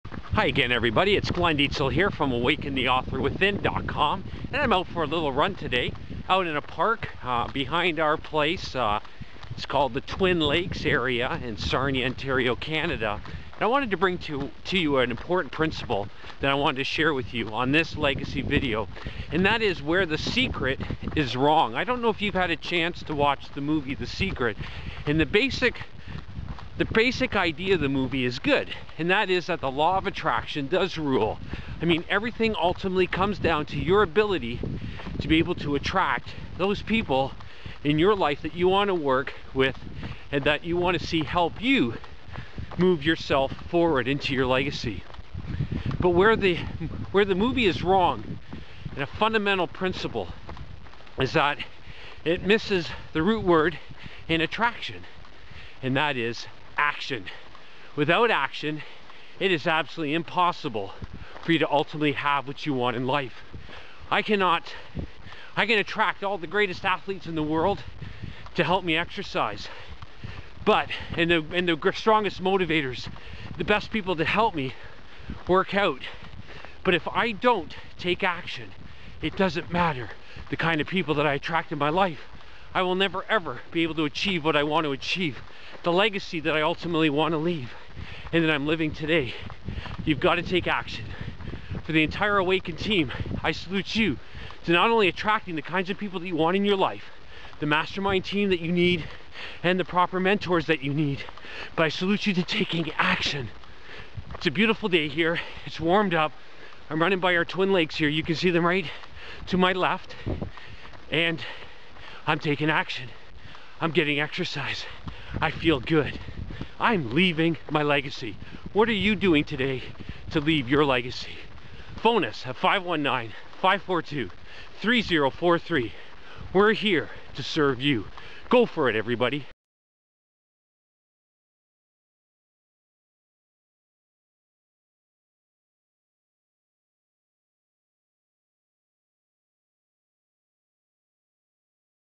Authoring Coach Shares How "The Secret" misses the boat.
Glenn Dietzel, authoring coach and speaker, shares how the movie "The Secret" fails to really show you the whole picture. Without Action you will not succeed.